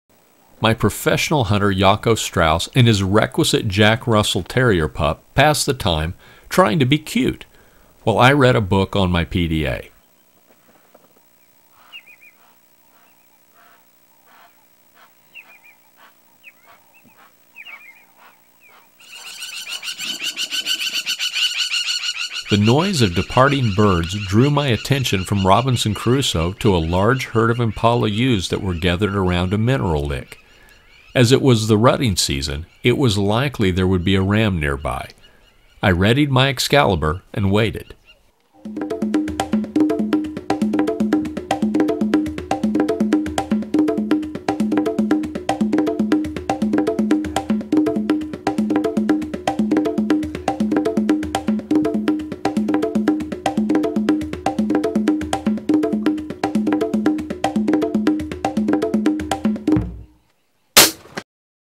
Safari Hunters Journal - Crossbow Impala
Venture to South Africa on a spot and stalk crossbow hunt.
3135